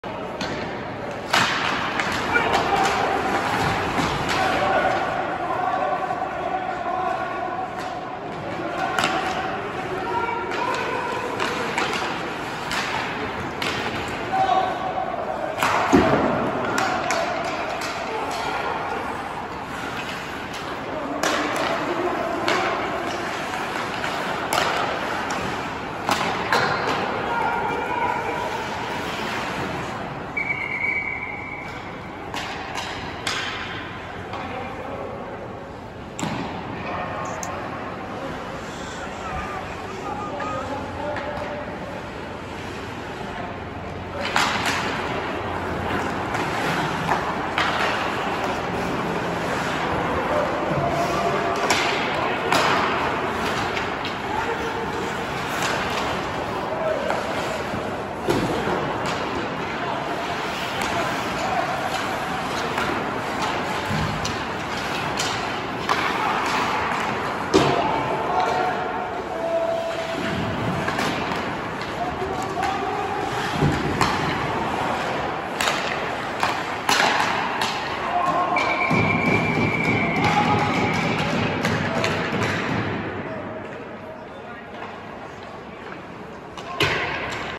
SeaKraken Power Play vs the Penalty Kill drill. 12-1
Morning skate work for the Seattle Kraken on the PP vs the PK. Both units. Kraken Community Iceplex.